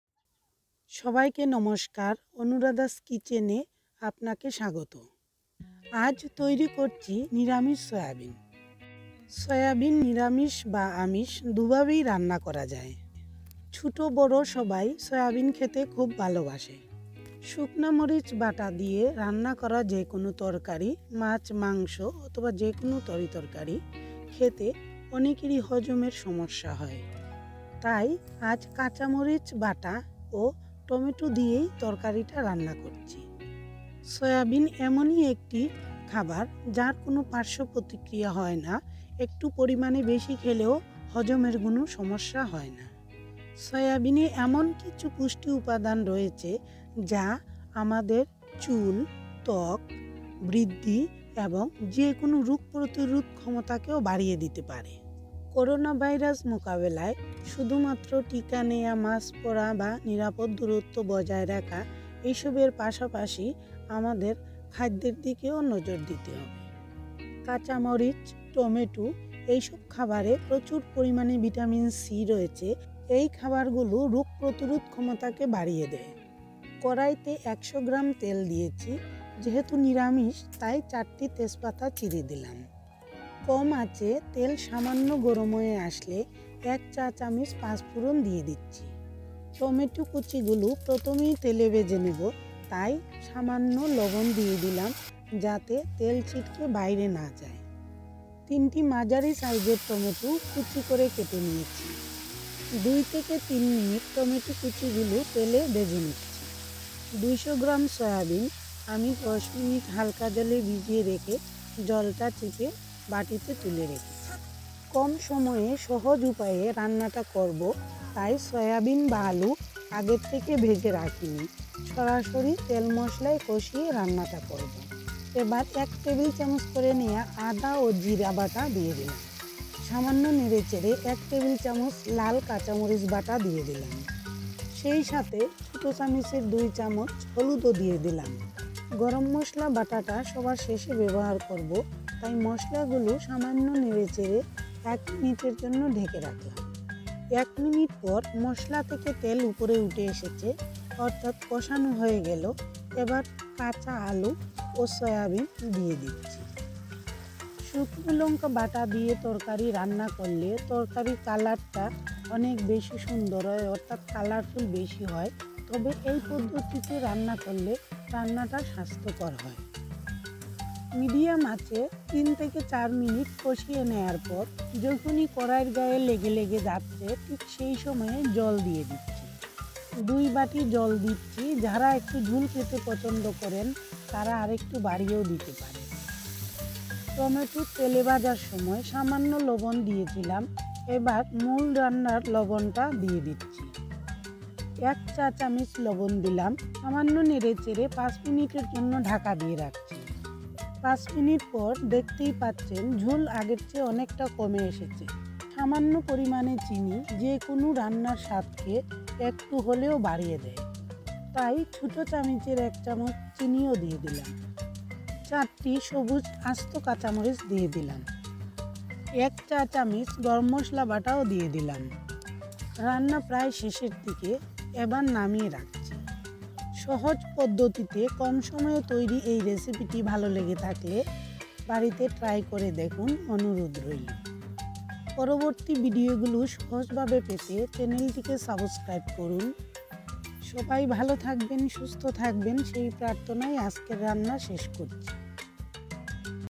নিরামিষ সয়াবিন এই ভাবে রান্না করলে বাচ্চা ও বড়রা আঙুল চেটে পুটে খাবে এবং প্রশংসা করবে
সয়াবিন নিরামিষ রেসিপি ।ভিটামিন সি সমৃদ্ধ এই রেসিপি রোগ প্রতিরোধ ক্ষমতা বৃদ্ধি করে ।এইভাবে সয়াবিন রান্না করলে বাচ্চারা খেতে পারে ।সয়াবিন খুবই পুষ্টিকর খাবার,এই খাবার    আমাদের রোগ প্রতিরোধ ক্ষমতা বাড়ায় ।এছাড়া ও বাচচাদের বুদ্ধি,বিকাশ ও হজম করতে সাহায্য করে।যাদের হজমের সমস্যা রয়েছে তাদের জন্য সয়াবিন খুবই ভালো।কাঁচা মরিচ ও টমেটো দিয়ে তৈরি করা এই রেসিপি স্বাদে অতুলনীয়।ছোট বড় সবাই কমবেশি এই খাবার খেতে ভালোবাসেন।কাঁচা মরিচ ও টমেটোতে ভিটামিন সি রয়েছে যা আমাদের দেহের জন্য অত্যন্ত উপকারী এবং রোগ প্রতিরোধ ক্ষমতা বৃদ্ধি করে ।করোনা ভাইরাস প্রতিরোধে এই রেসিপি খুবই  কার্যকর।সয়াবিন কম সময়ে তৈরি করা যায়,খেতে ও ভালো।যাদের হজমের সমস্যা রয়েছে তাদের জন্য সয়াবিন খুবই ভালো ।শুকনো মরিচ দিয়ে রান্না করলে অনেকেরই পেটের সমস্যা হয়।তাই কাঁচা মরিচ দিয়ে রান্না করলে খেতে দারুণ লাগে এবং হজমের সমস্যা ও হয় না ।কম সময়ে তৈরি করা এই রেসিপি স্বাদে অতুলনীয় এবং রোগ প্রতিরোধ ক্ষমতা বৃদ্ধি করে।বাড়িতে মাছ ও মাংস বা ডিম,ডাল কিছু না থাকলে এভাবে সয়াবিন রান্না করলে সবাই আঙুল চেটে পুটে খাবে।লোভনীয় স্বাদে তৈরি এই রেসিপি রুটি,পরটা অথবা গরম ভাতের সাথে আর কিছুই লাগবে না।কম খরচে দুর্দান্ত স্বাদের এই রেসিপি স্বাদে অতুলনীয় এবং স্বাস্থ্যকর।আলু দিয়ে রান্না করলে খেতে বেশি স্বাদ লাগে।নিরামিষ সয়াবিন নারকেলের দুধ দিয়ে রান্না করলে খেতে আরো ভালো লাগবে।নারকেলের দুধ গরুর দুধের মতো অনেক বেশি পুষ্টিকর ও ভিটামিন রয়েছে।নারকেলের দুধ খেয়ে অনেকেরই গ্যাসটিক হয় তাদের জন্য উপযুক্ত না।কম তেল সামান্য বাটা মসলা দিয়ে রান্না করলে ভালো হয়।